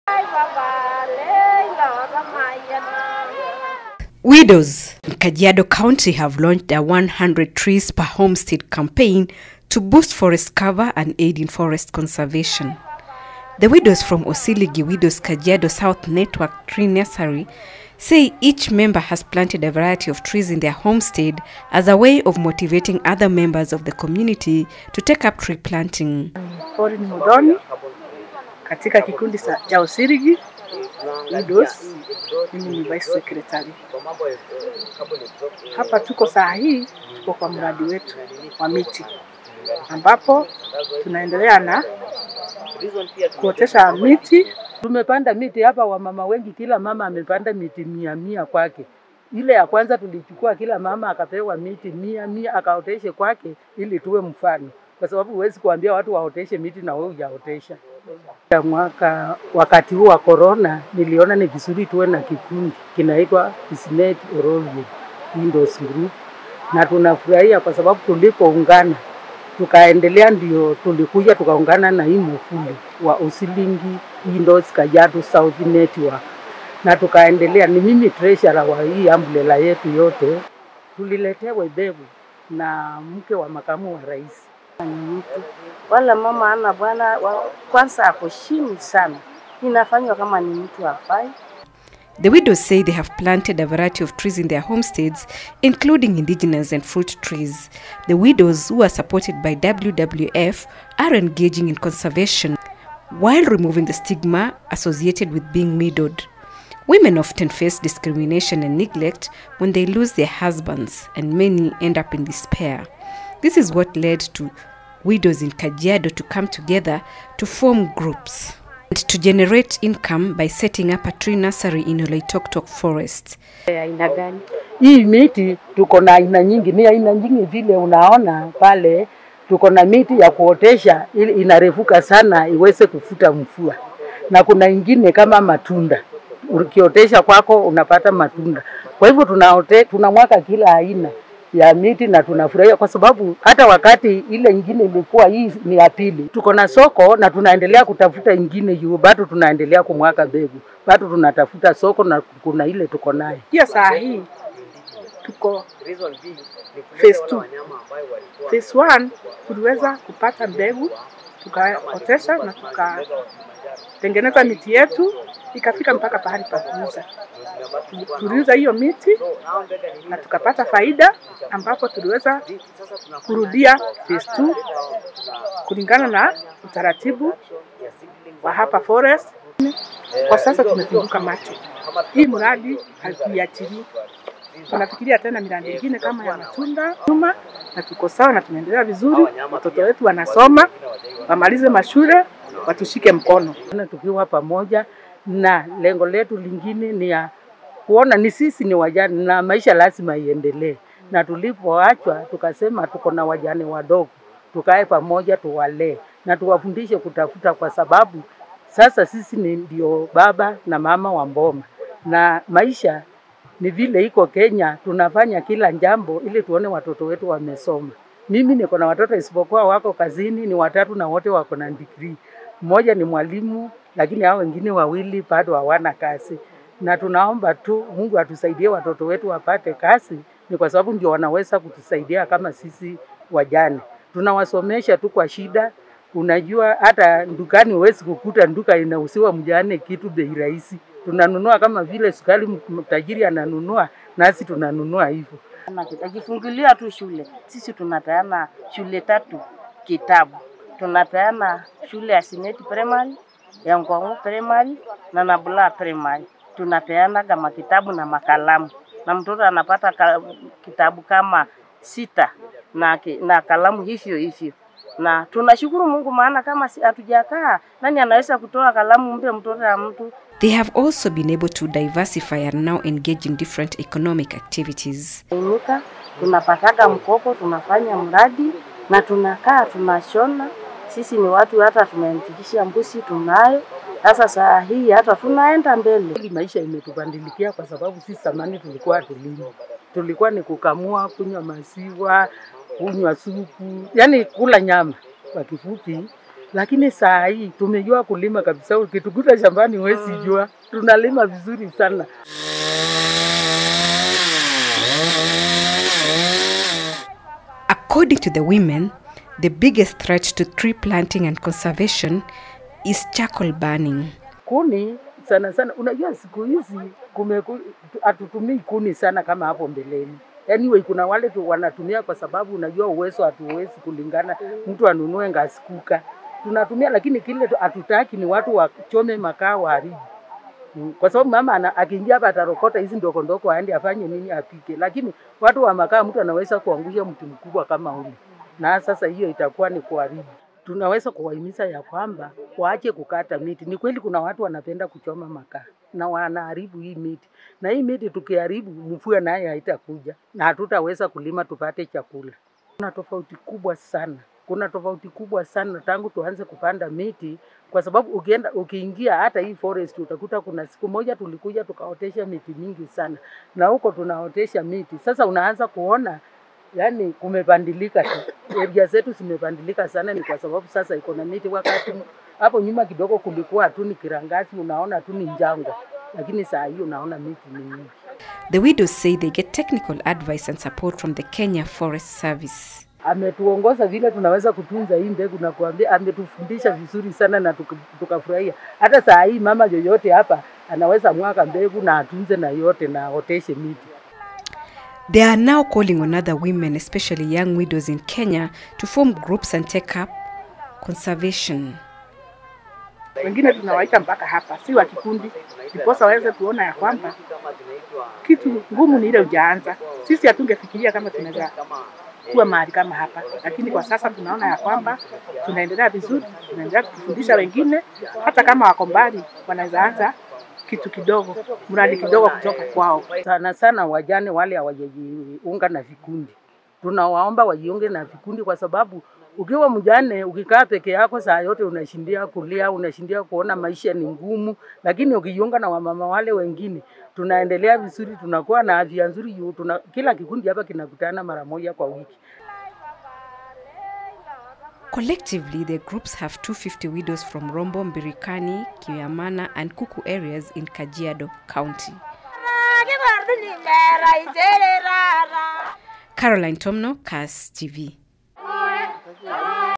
WIDOWS TAKING LEAD IN CONSERVATION
In their communities, women are coming up with creative solutions to combat climate change. To increase the amount of forest cover, a few women's groups are urging the residents of their communities to plant at least 100 trees in their backyards.